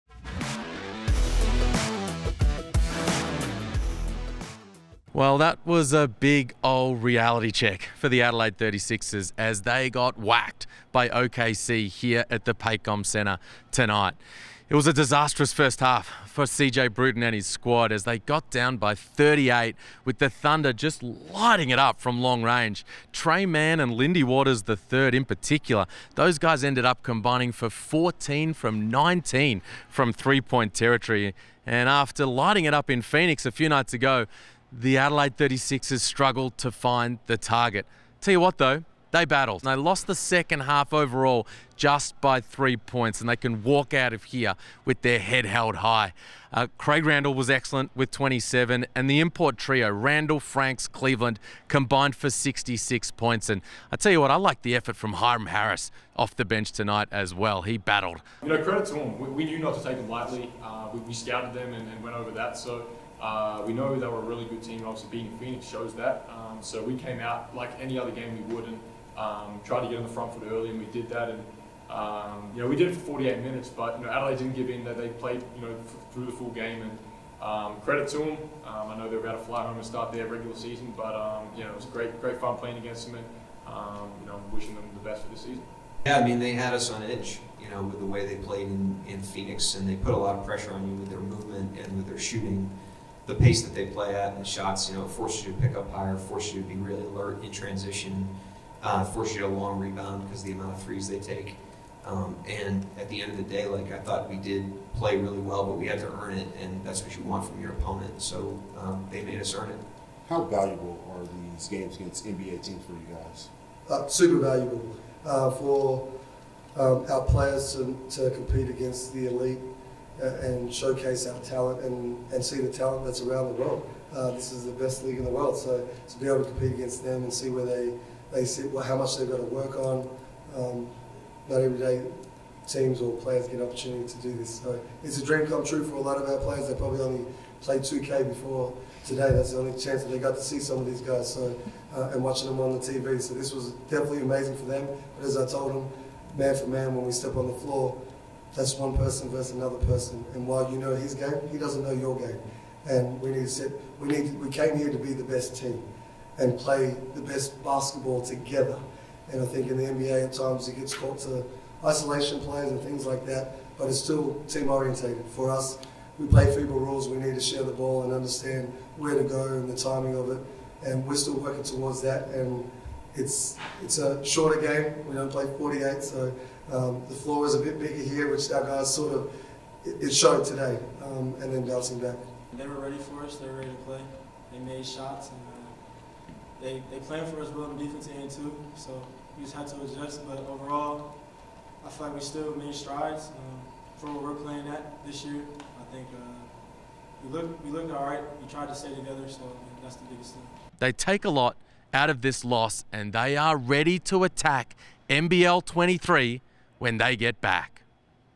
Adelaide 36ers vs Oklahoma City Thunder Post Game Wrap Up (NBLxNBA, 2022)
Liam Santamaria wraps up the Adelaide 36ers NBLxNBA experience

Josh Giddey, Mark Daigneault, CJ Bruton and Craig Randall II chat to the media post game.